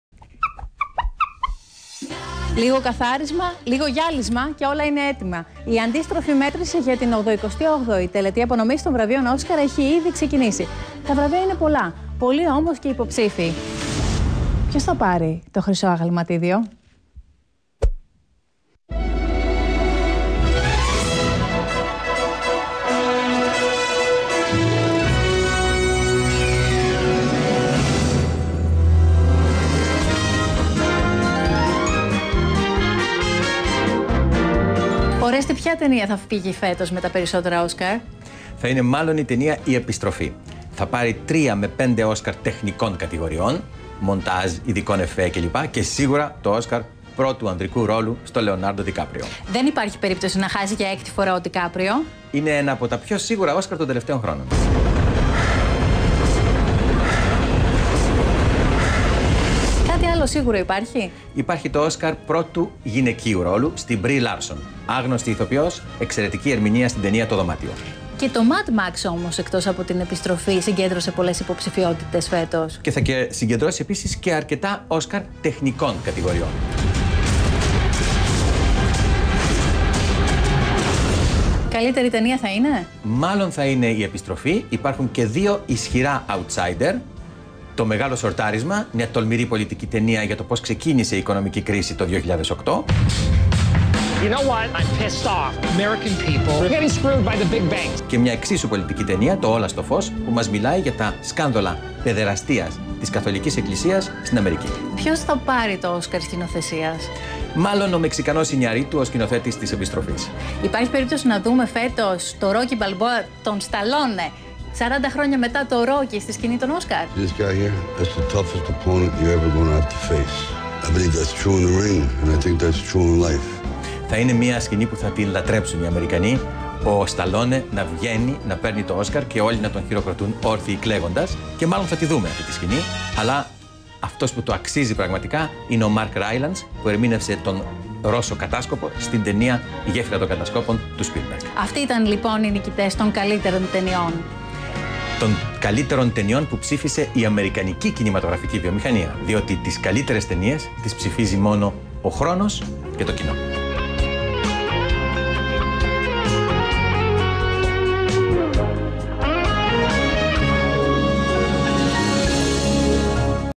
Όλο το παρασκήνιο πριν από την αυριανή απονομή των Oscars - MEGA ΓΕΓΟΝΟΤΑ
Λίγες ώρες απομένουν, έως τη μεγάλη βραδιά του Χόλυγουντ και την απονομή των βραβείων Οσκαρ, και ήδη τα στοιχήματα πέφτουν βροχή για τους φετινούς νικητές.